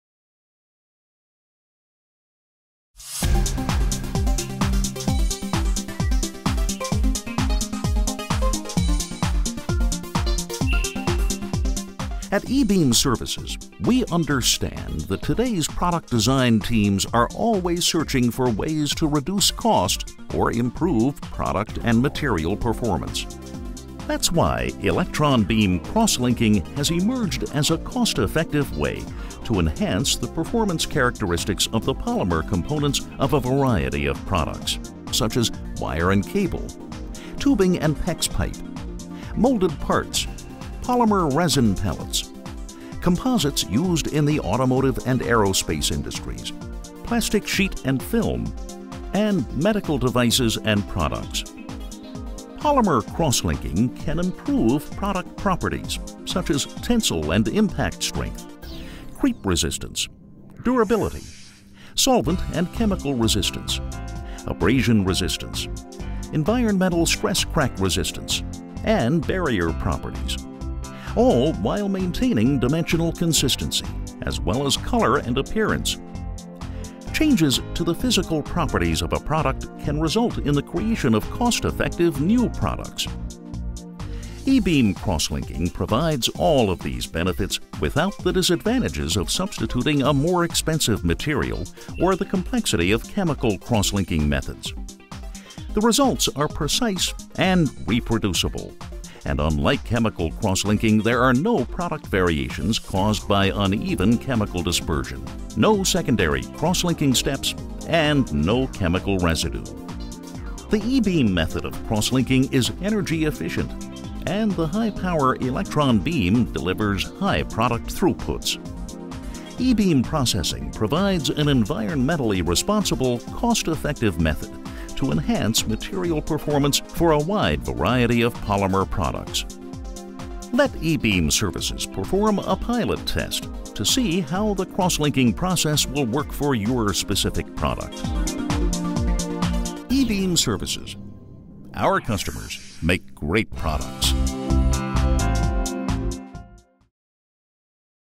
Enhancing Polymers with E-Beam
Electron beam crosslinking has emerged as a cost-effective way to enhance the performance of plastic wire and cable, tubing and pex pipe, molded parts, polymer resin pellets, composites, plastic sheet and film and medical devices.